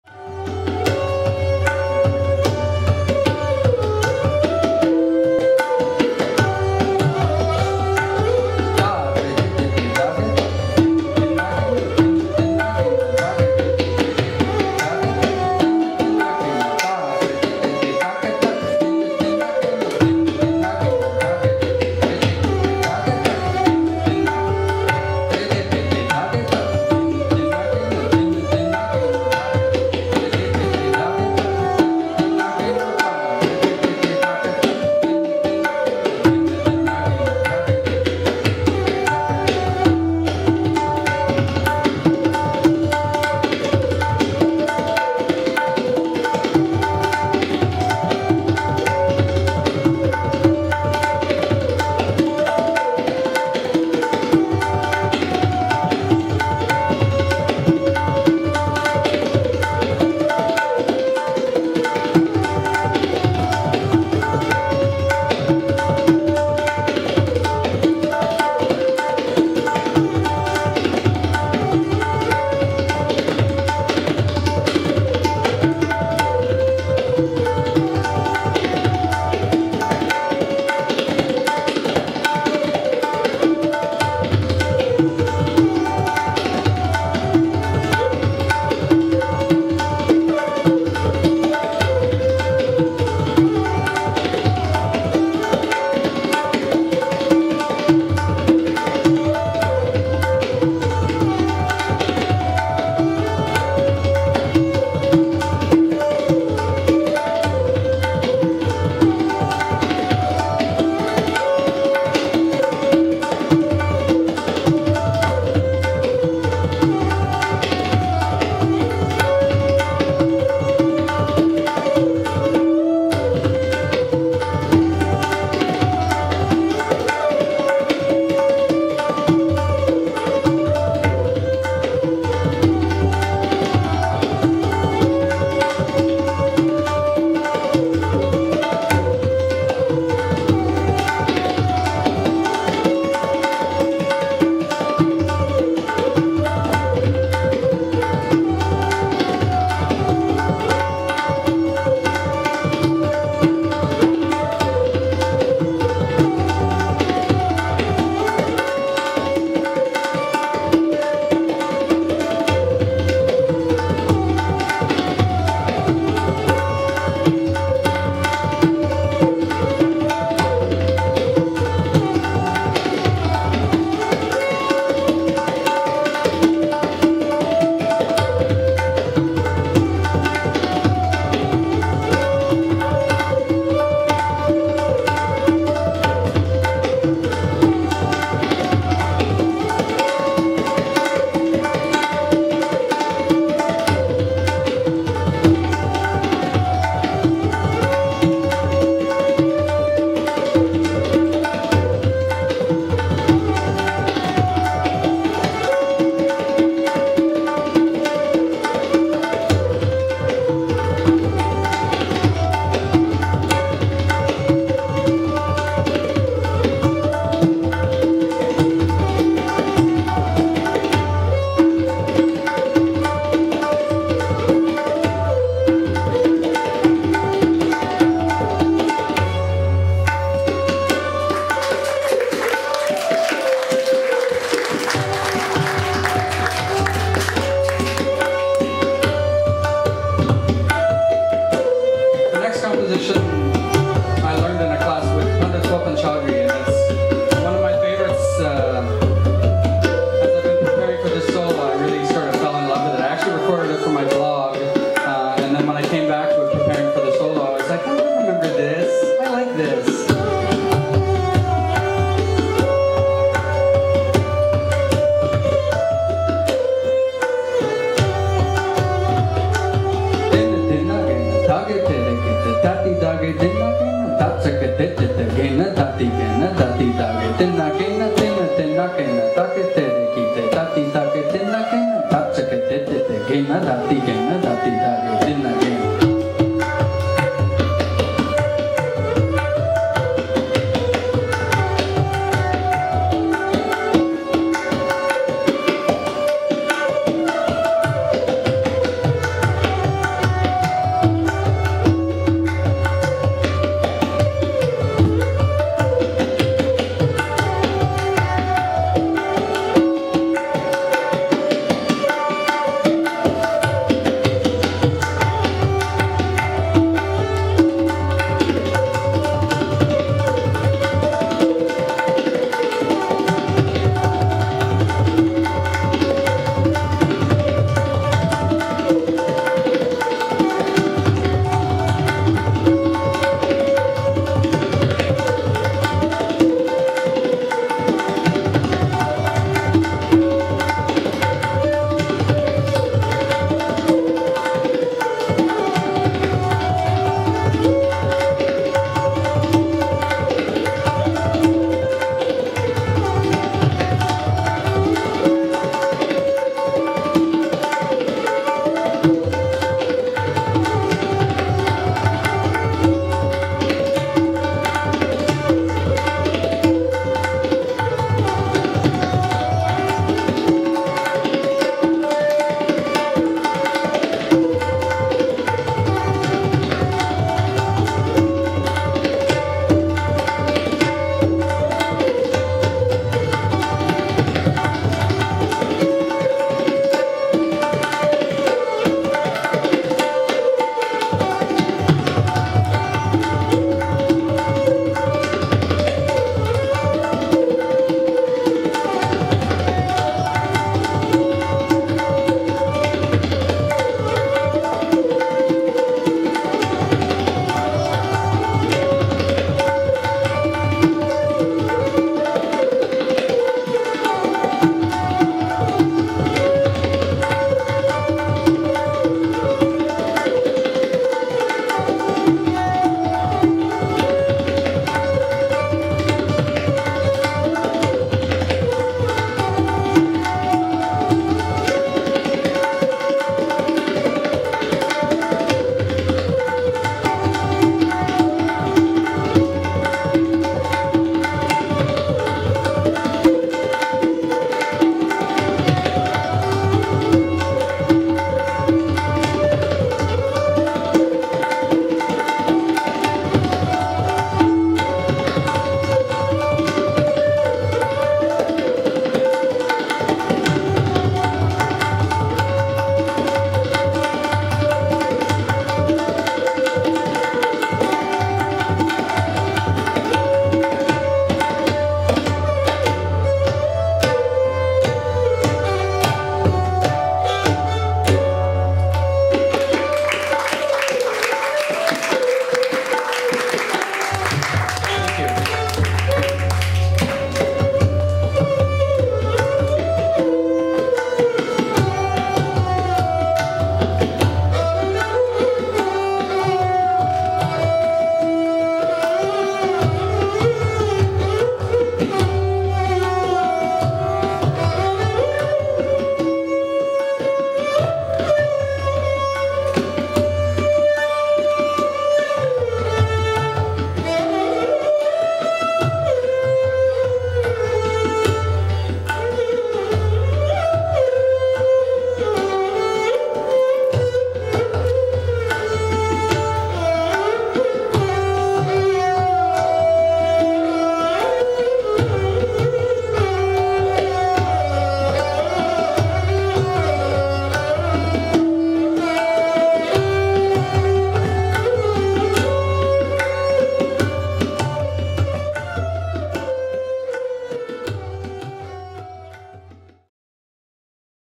Ed Hanley: Tabla Solo, Jhaptal, Part 4
Recorded live in concert on August 21, 2012 at Musideum in Toronto, Canada.

Ed Hanley-Tabla
Rattan Bhamrah-Esraj

Tala (rhythm cycle): Jhaptal (10 beats)

A Kaida-Rela I learned from Subhajyoti Guha, and a Kaida I learned from Swapan Chaudhuri.

I will post the entire concert in sections.

Camera-Stephen Sirisko
Video editing-Ed Hanley

Liner notes for this video:
...are coming...